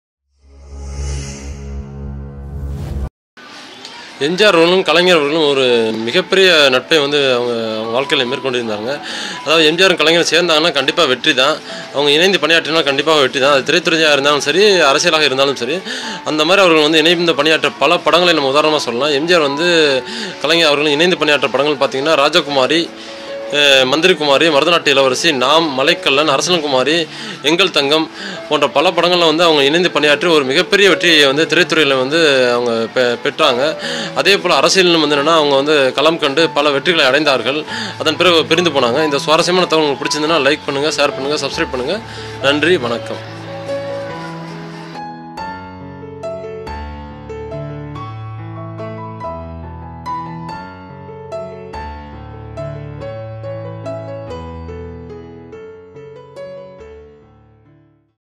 MGRயாரும் கலைஞரும் இணைந்து வெற்றி வாகை சூடிய தமிழ் திரை காவியங்கள் Tamil Films that MGR & Kalaignar
In this video, detailed about the list of Tamil films which the legendary Actor politician MG Ramachandran and Kalaignar M Karunanidhi worked together.